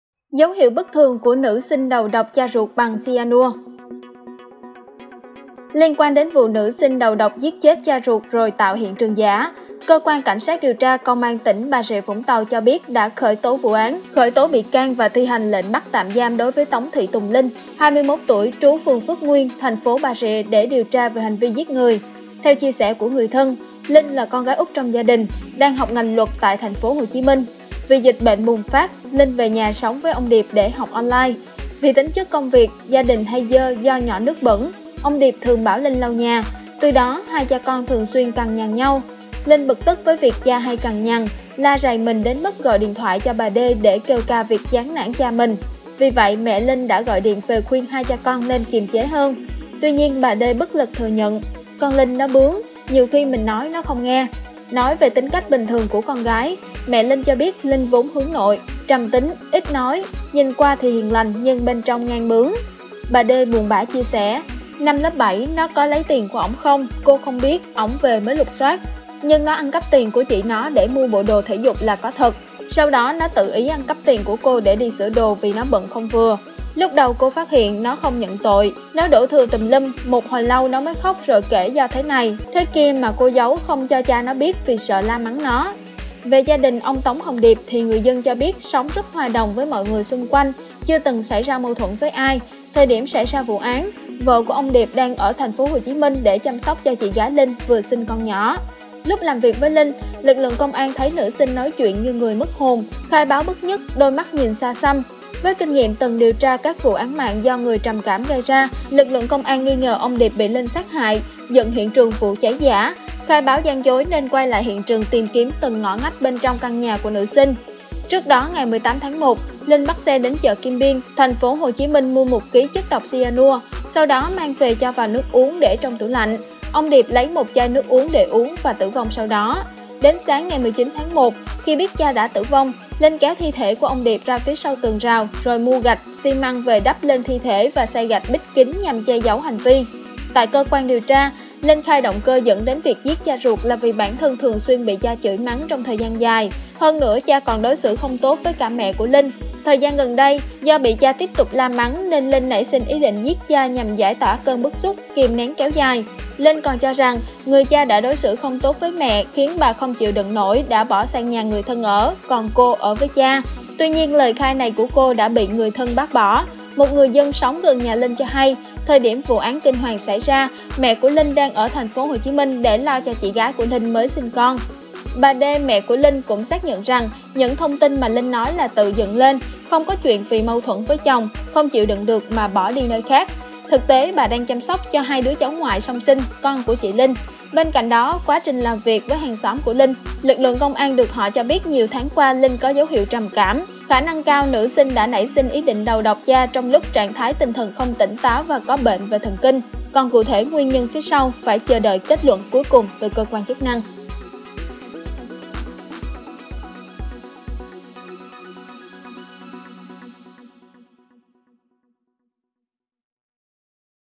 🔥 Dấu hiện "bất thường" của nữ sinh đầu độc cha ruột bằng xyanua: Dựng chuyện trắng trợn
Nói về tính cách bình thường của con gái, mẹ Linh cho biết Linh vốn hướng nội, trầm tính, ít nói, nhìn qua thì hiền lành nhưng bên trong ngang bướng. Lúc làm việc với Linh, lực lượng công an thấy nữ sinh nói chuyện như người mất hồn, khai báo bất nhất, đôi mắt nhìn xa xăm.